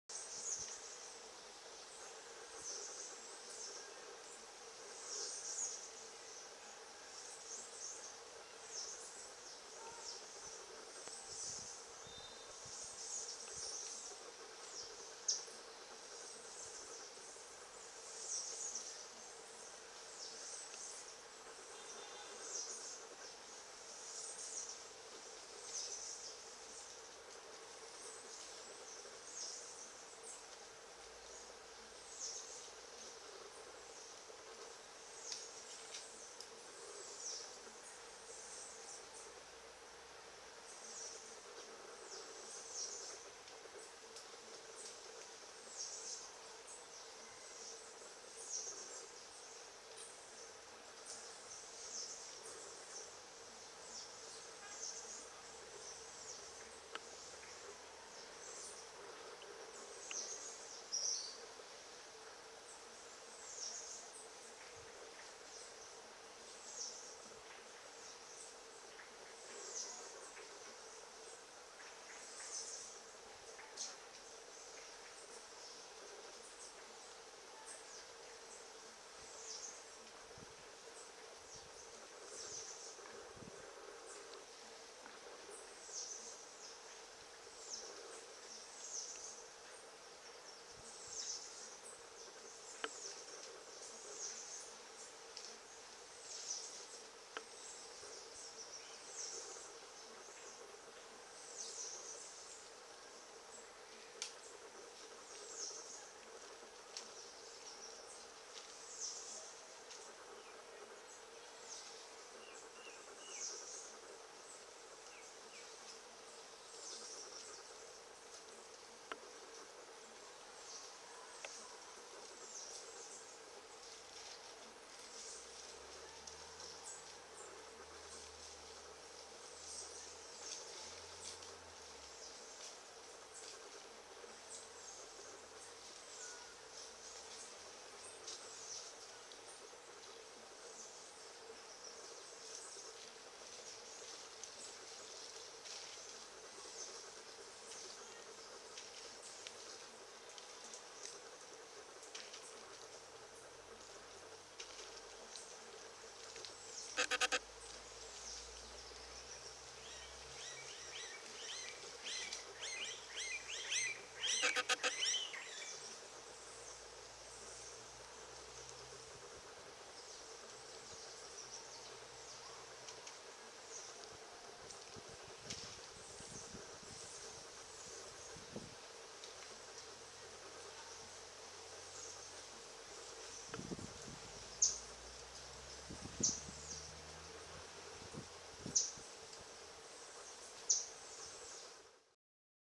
Lizard having avocado for lunch
Dominican ground lizard having avocado for its lunch